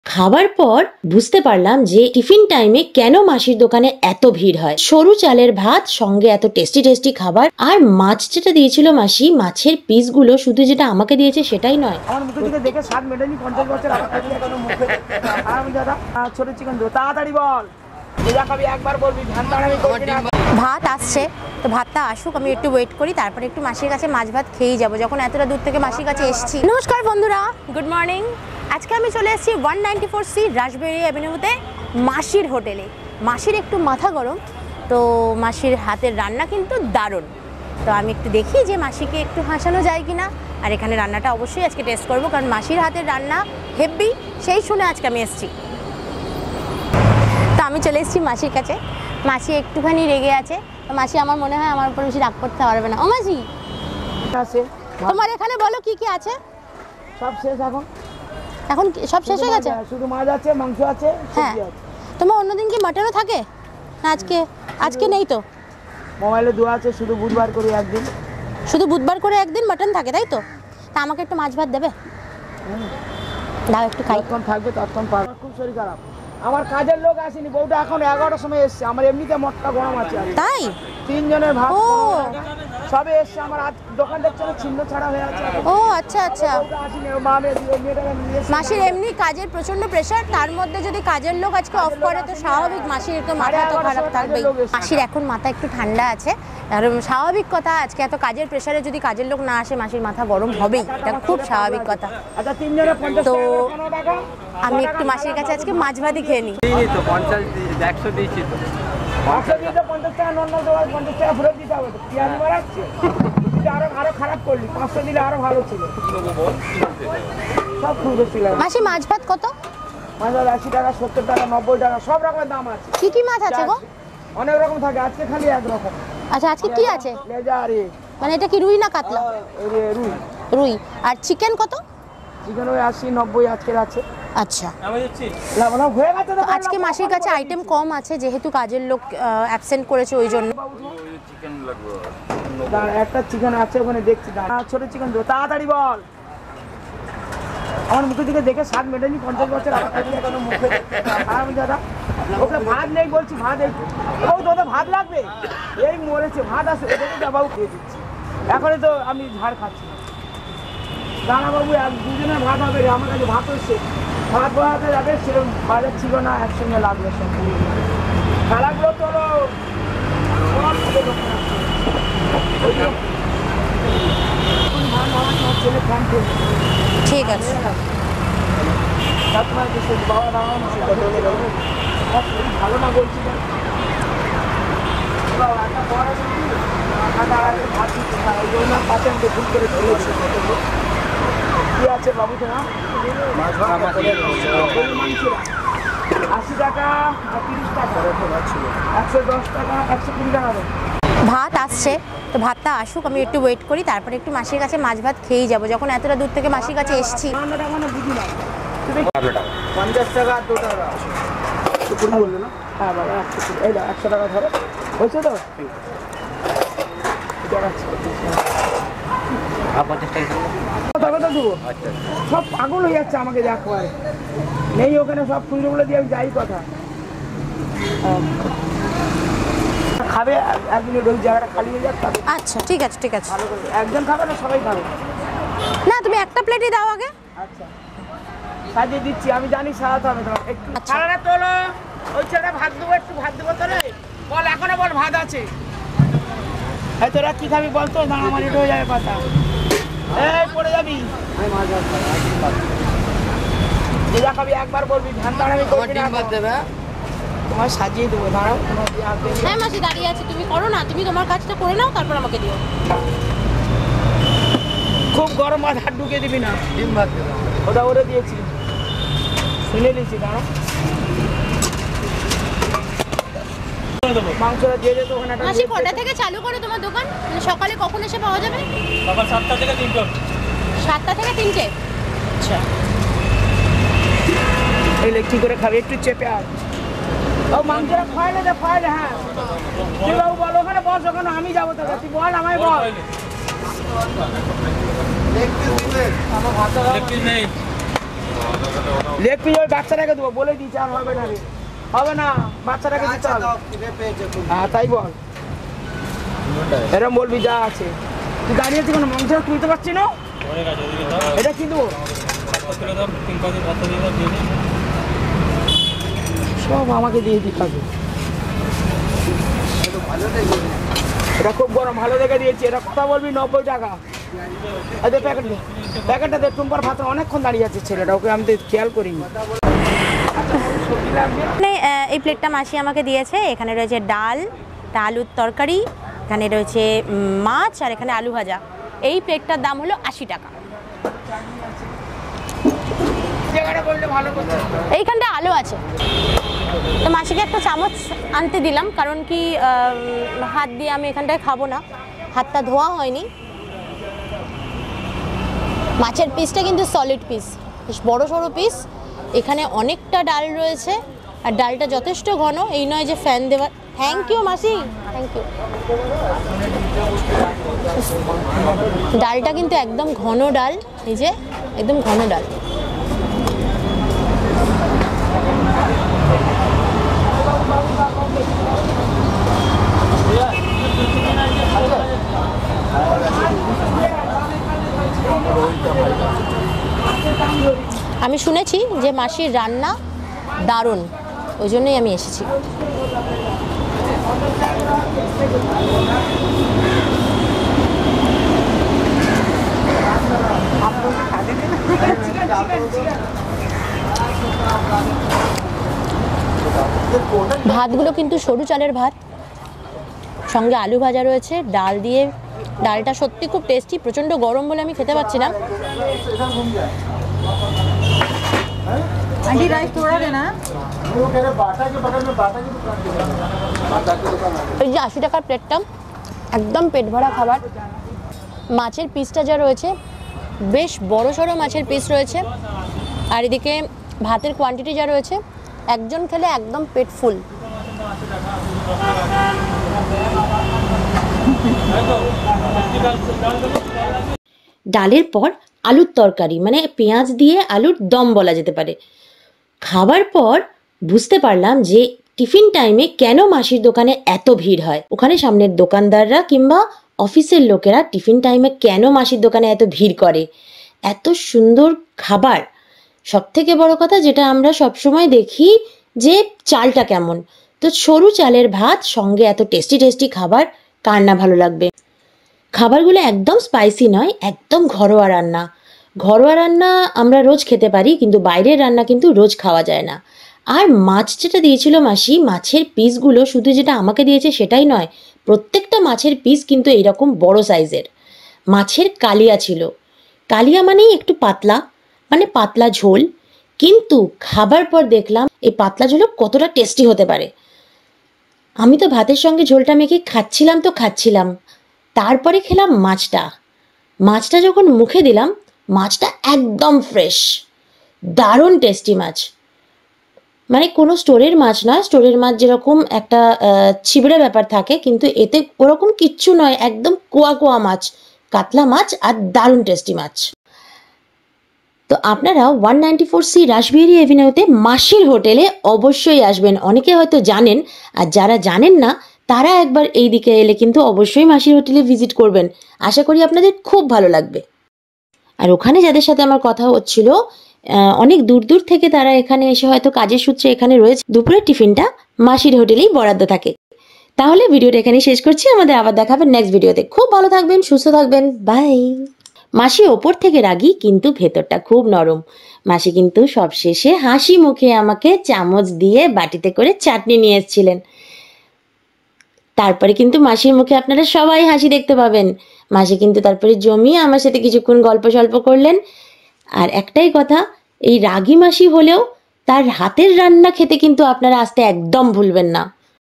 Masi Ragi Hole Ki Hobe | Masir Ranna Apurba | 80 Takai Mach Bhaat ( Rice ) Thali | Ragi Masir Hotel
Address :  194c, Rash Behari Ave, Golpark, Hindustan Park, Gariahat, Kolkata, West Bengal 700029 .

Masi Ragi Hole Ki Hobe | Masir Hater Ranna Apurbo | 80 Takai Mach Bhaat ( Rice ) Thali | Ragi Masir Hotel